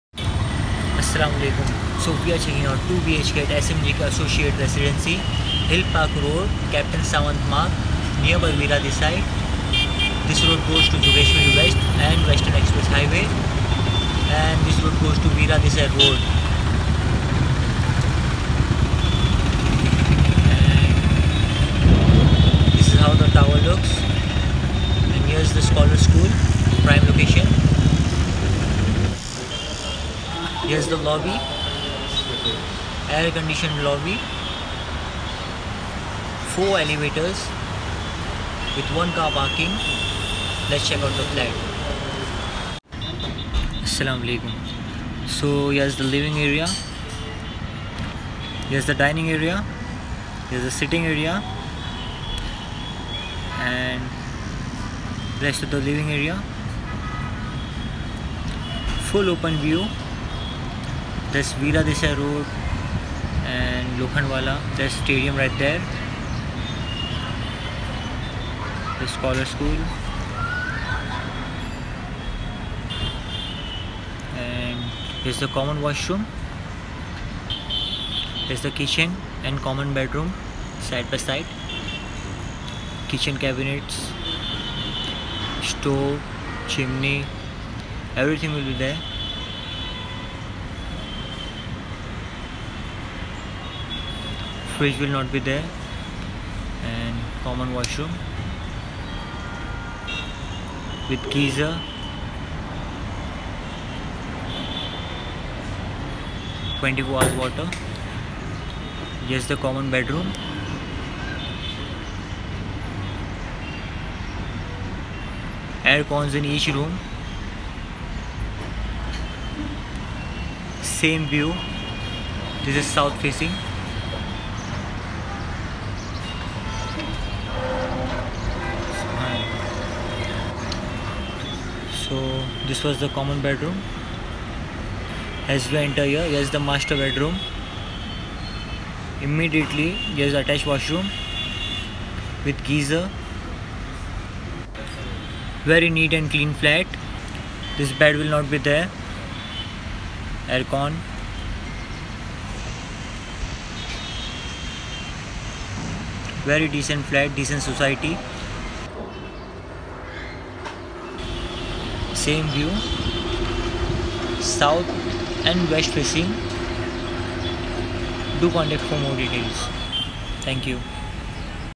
2bhk SMGK Associate Residency, Jogeshwari west
Semi Furnished flat.
Easy access to Prime Location like Lokhandwala, Azad Nagar, Western Express Highway etc.